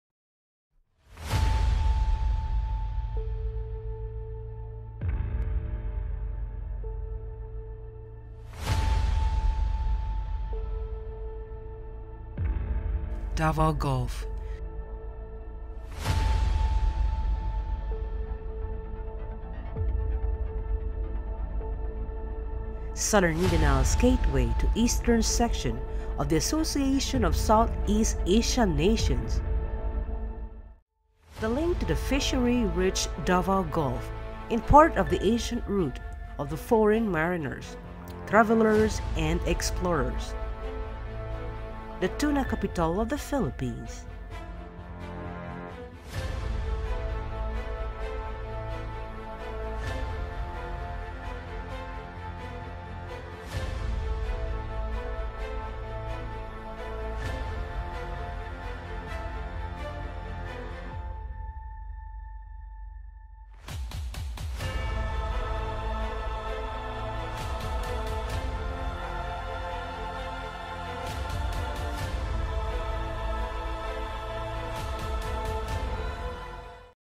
Davao Gulf - Teaser
Journal of Davao Narratives teaser on the Tuna Capital of the Philippines - the Davao Gulf.
Join the journey in discovering the history of Davao Region.

Jojie Alcantara Photography